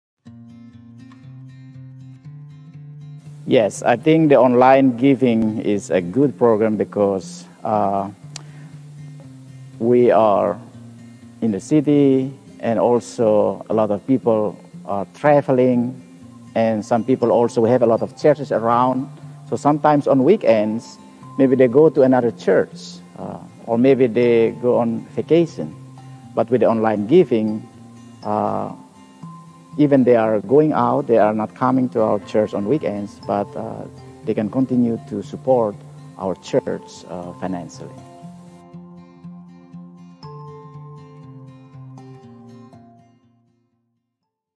Online Giving: Rev. Emanuel R. Tanu, SVD Testimonial
Today’s parishioners are on the move and are used to
having many options to engage and communicate.
They need flexibility in order to engage and contribute.
Today’s parish can serve those needs with Online Giving
from OSV. Online Giving provides a convenient way
for parishioners to make financial contributions, and
it relieves church staffs from managing secure bank
account information at the same time. Administration is
simple and easy. Online Giving automatically integrates
with OSV Church Manager and can export data for
updating records in virtually any parishioner data
management system so contributions are recorded and
attached to parishioner records.